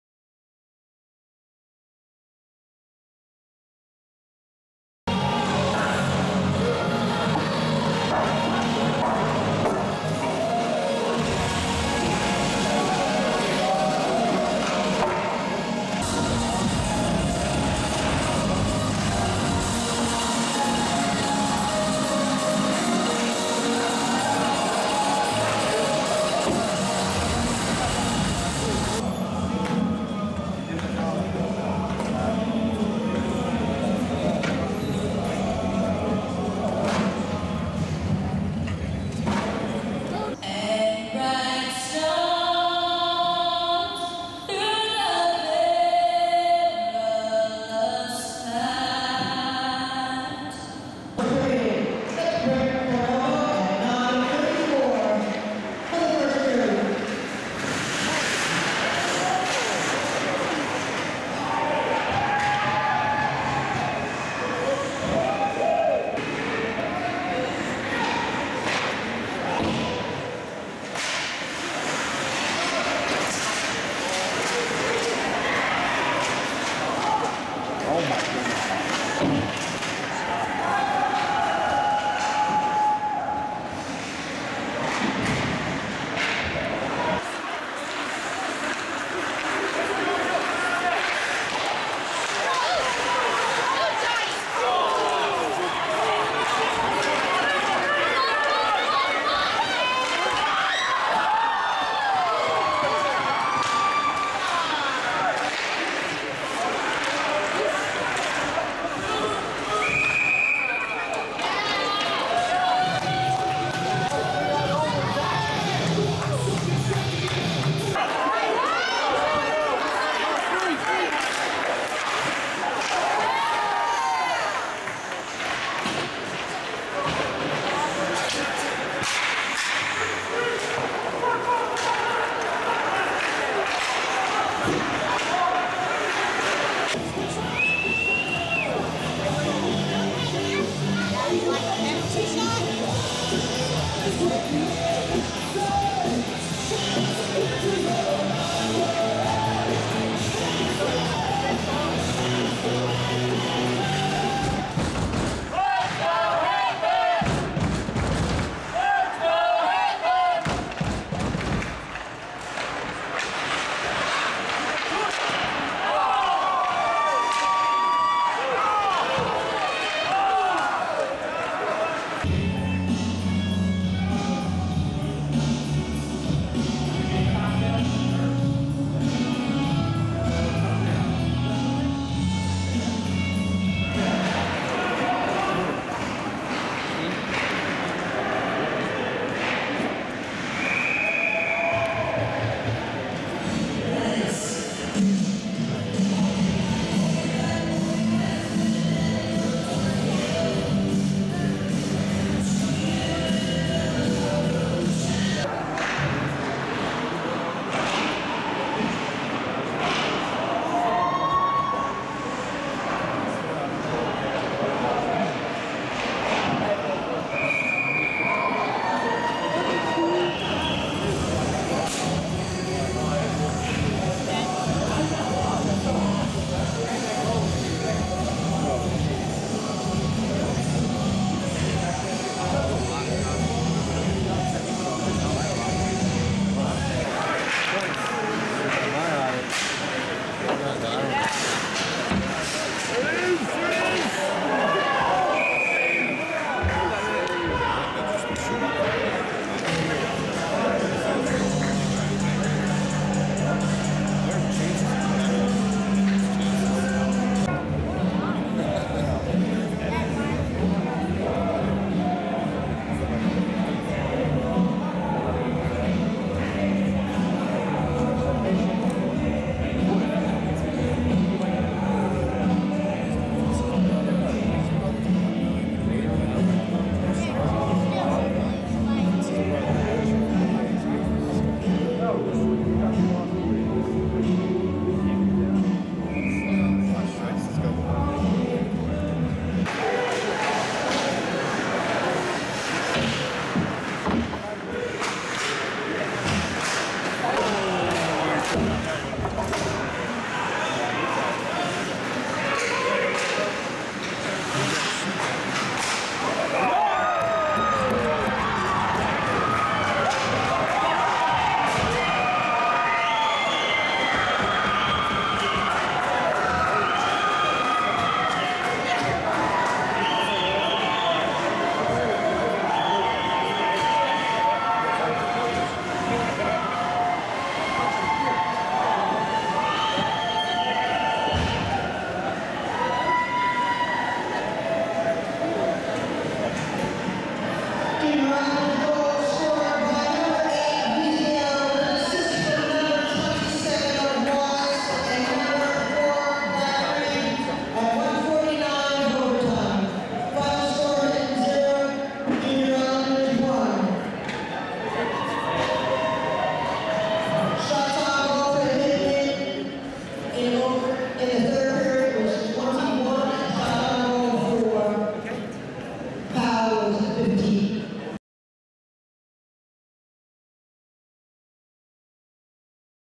PAL Jr. Islanders-Jersey Hitmen Highlights, January 4, 2015
USPHL action from the Ice Vault, Wayne New Jersey. JUSTIN KETOLA recorded 55 saves en route to his first shutout of the season and BILLY VIZZO scored the only goal of the game in overtime as the Jr. Islanders record a 1-0 road victory.